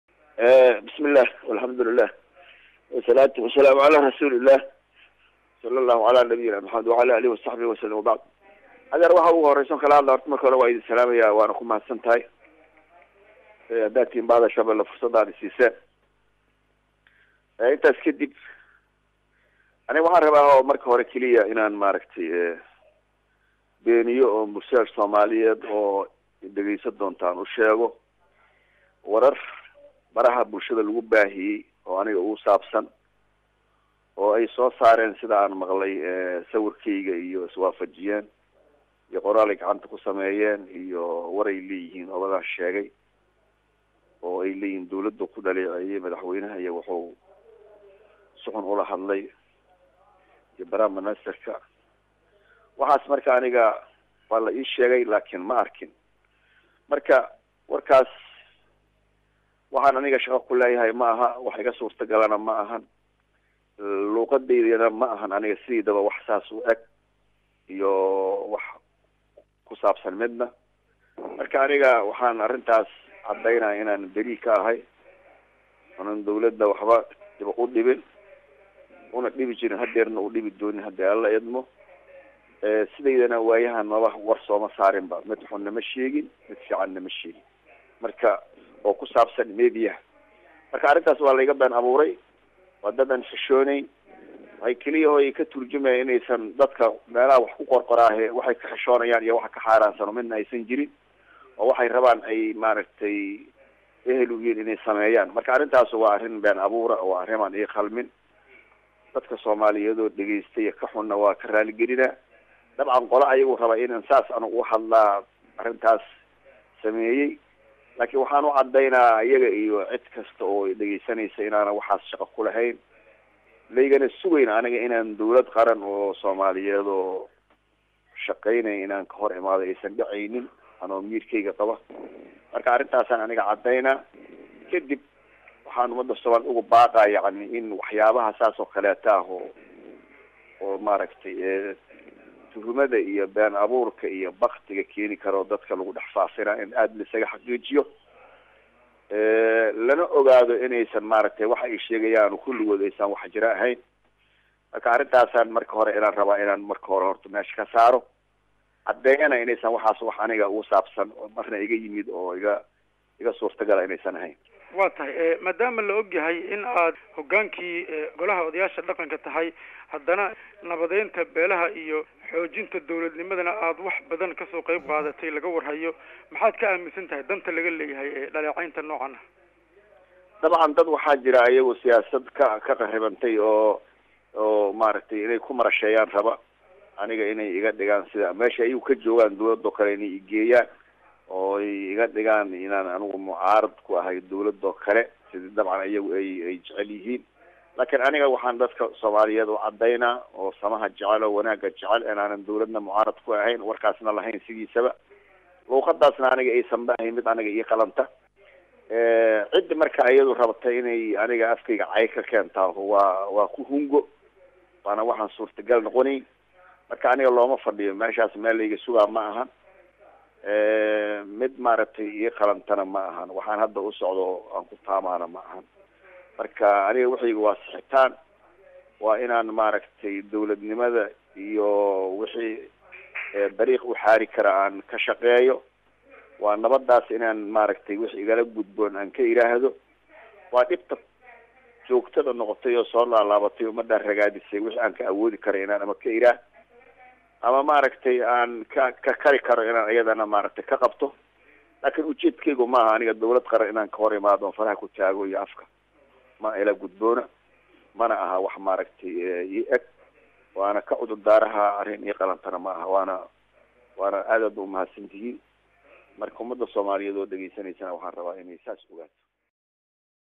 DHAGEYSO:-Nabadoon Xaad oo iska Fogeeyay Warar laga Faariyay
Gudoomiyaha Golaha dhaqanka iyo midnimada Beelaha hawiye Xaaji Maxamed Xasan Xaad oo ka hadlay warar shaqsiyadiisa lagu waxyeeleeyay oo lagu faafiyay warbaahinta ayaa jawaab u diray shaqsiyaadka ku howlan arintaasi,